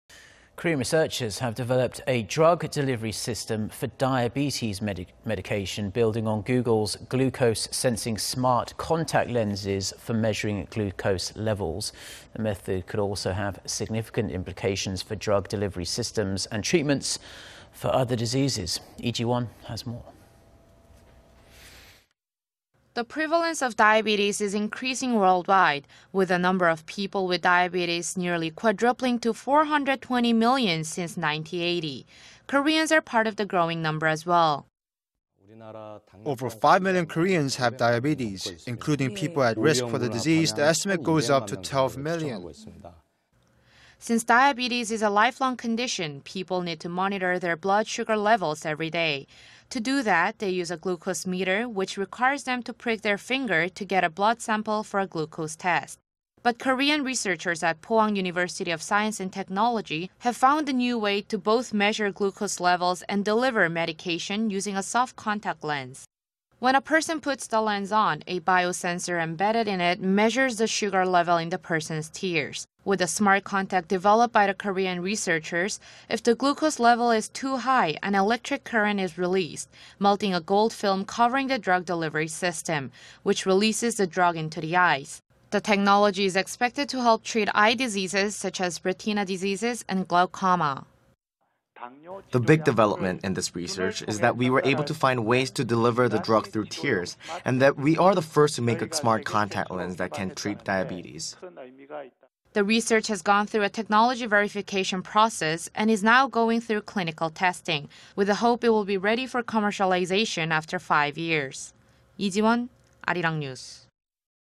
Korean researchers develop drug delivery system for diabetes medication
렌즈로 혈당 측정하고 약물 투여까지...세계 최초 개발
Korean researchers have developed a drug delivery system for diabetes medication,... building on Google's glucose-sensing smart contact lens for measuring glucose levels.
The method could also have significant implications for drug delivery systems and treatments for other diseases.
Lee Ji-won has more.
The prevalence of diabetes is increasing worldwide, with the number of people with diabetes nearly quadrupling to 420-million since 1980.
Koreans are part of the growing number as well.

"Over 5-million Koreans have diabetes. Including people at risk for the disease, the estimate goes up to 12-million."

Since diabetes is a lifelong condition, people need to monitor their blood sugar levels every day.
To do that, they use a glucose meter, which requires them to prick their finger to get a blood sample for a glucose test.
But Korean researchers at Pohang University of Science and Technology have found a new way to both measure glucose levels and deliver medication using a soft contact lens.

When a person puts the lens on, a biosensor embedded in it measures the sugar level in the person's tears.
With the smart contact developed by the Korean researchers, if the glucose level is too high, an electric current is released, melting a gold film covering the drug delivery system,... which releases the drug into the eyes.
The technology is expected to help treat eye diseases such as retina diseases and glaucoma.

"The big development in this research is that we were able to find ways to deliver the drug through tears, and that we are the first to make a smart contact lens that can treat diabetes."

The research has gone through a technology verification process and is now going through clinical testing, with the hope it will be ready for commercialization after five years.